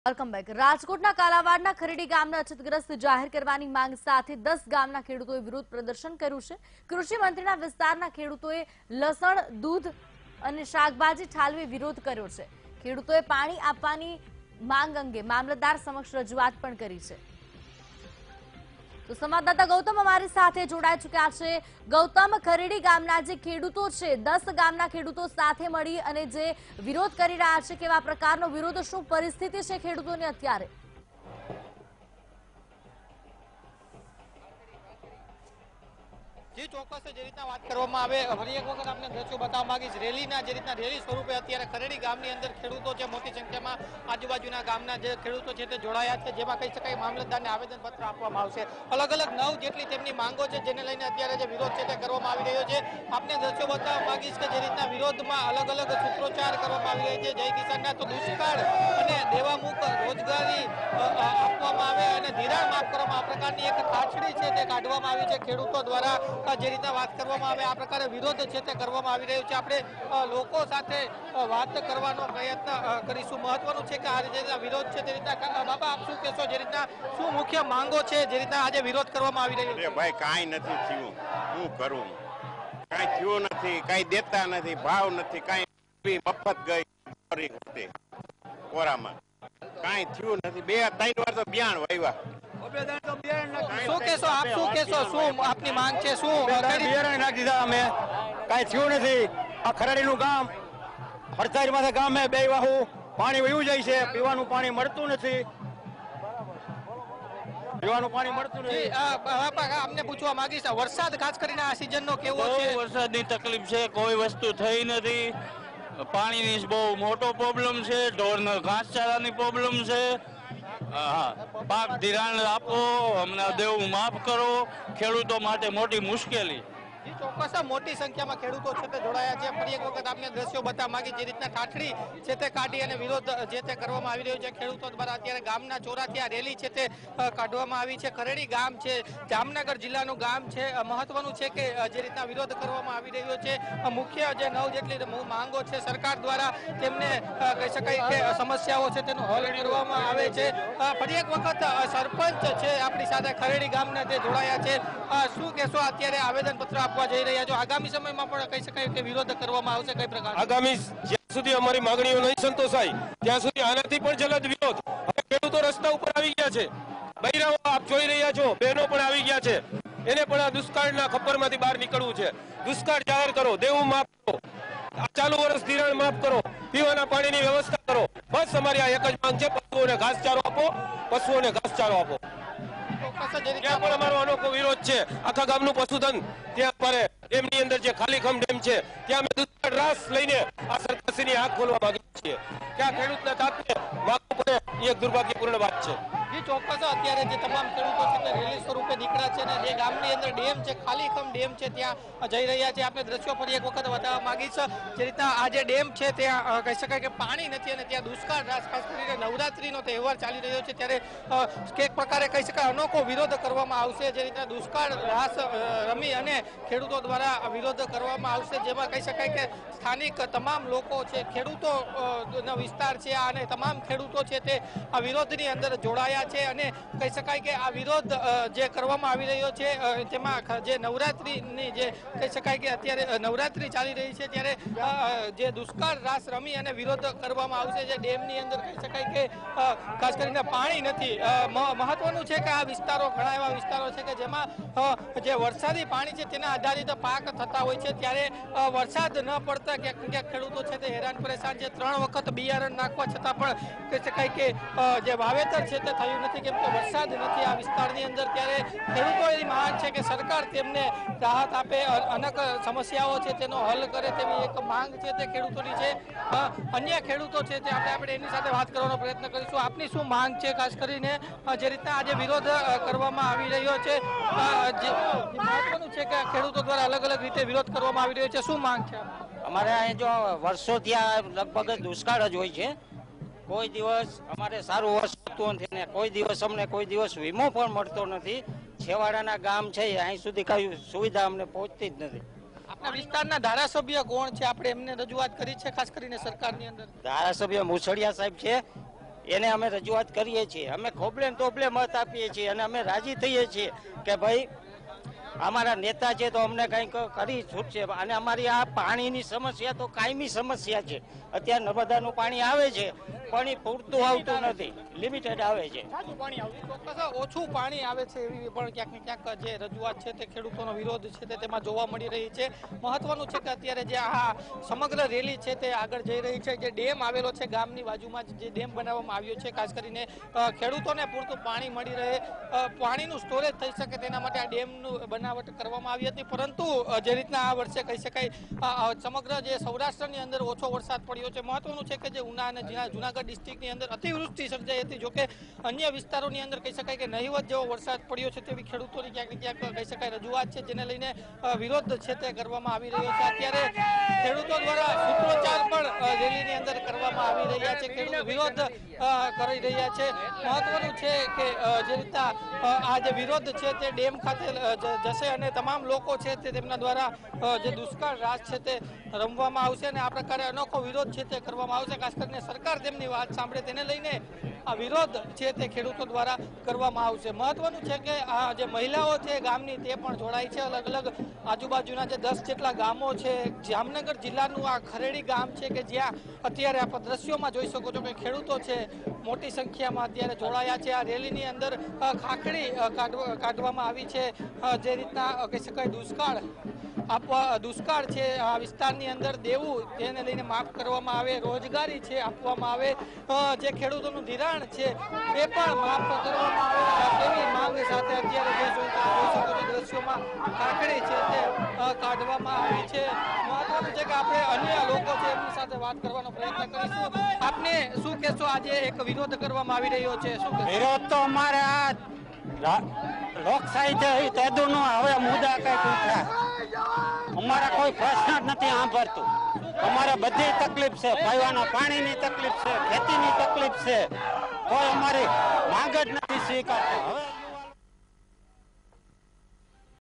Jamnagarના ખેડૂતોનું વિરોધ પ્રદર્શન, ખાલી પડેલા Khardi Damમાં કરશે દેખાવો | Vtv News
Jamnagarના ખેડૂતોનો વિરોધ પ્રદર્શન, ખાલી પડેલા Khardi Damમાં કરશે દેખાવો